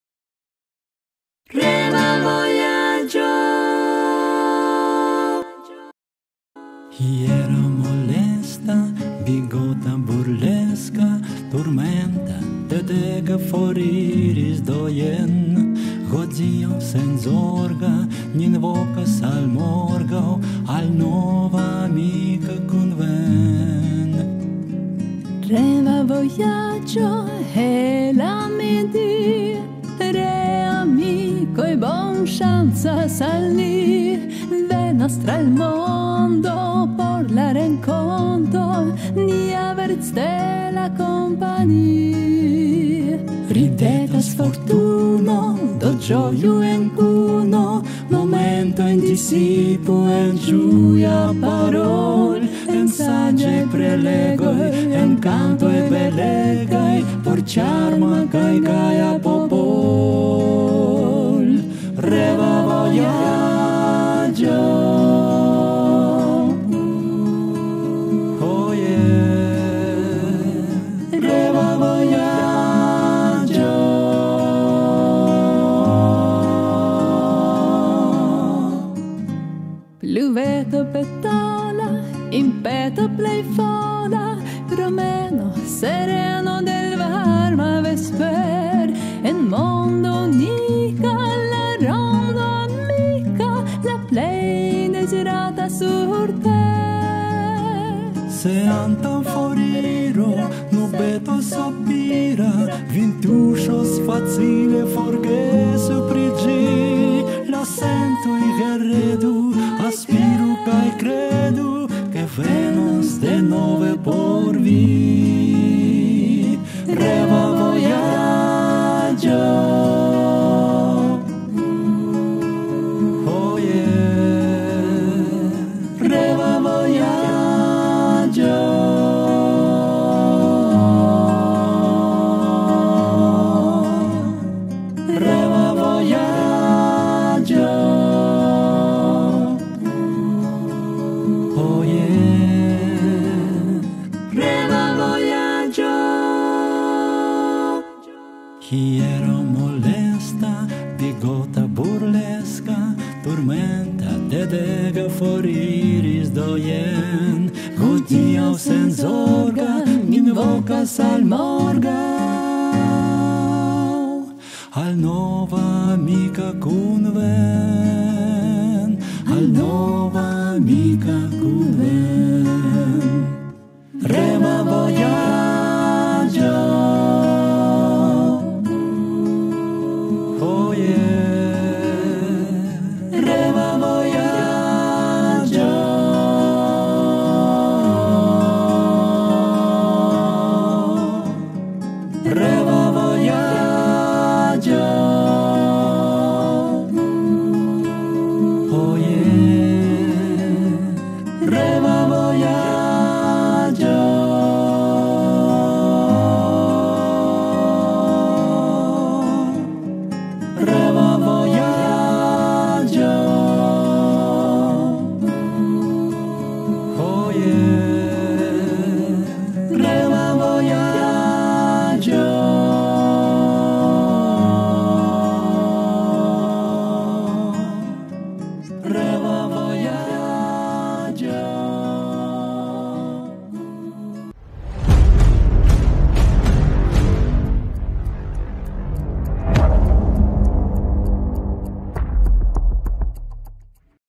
REVA VOJAGXO - Esperanta muziko | Путешествие мечты | Music Show 81
En ĉi tiu transdono ĉe Instinkto, la muzika spektaklo REVA VOJAGXO - Esperanta muziko.
В этом выпуске на канале Инстинкт музыкальное шоу "Путешествие мечты" - музыка эсперанто

Kiel la instinkta kanalo, ŝati kaj aboni